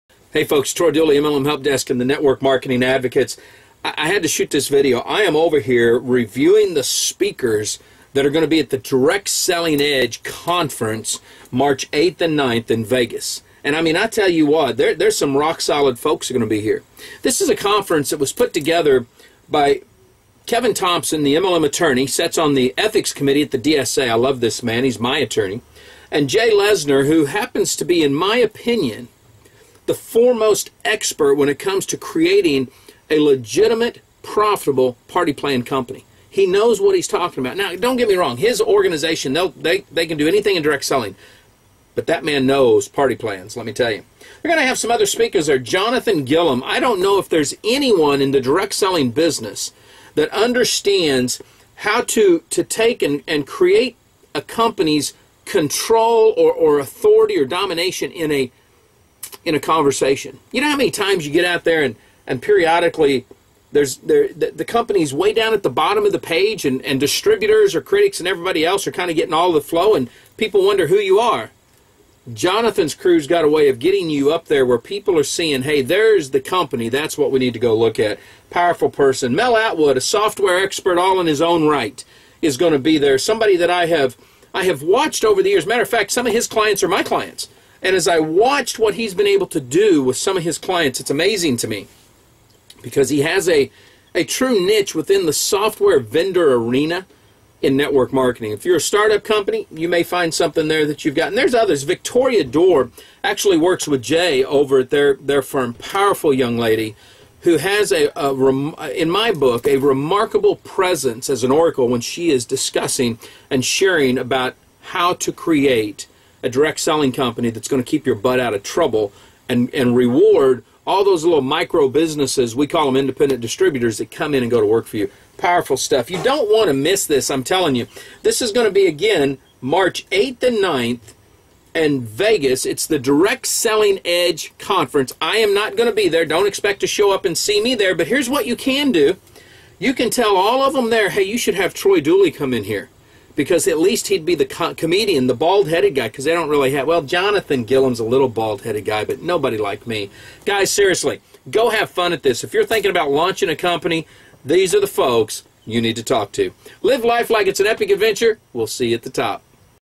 The Direct Selling Edge Conference Is Taking Network Marketing By Storm
MLM Attorney Kevin Thompson & Party Plan Consultant Jay Lienser have made direct selling history with their new direct selling conference for MLM and Party Plan startups.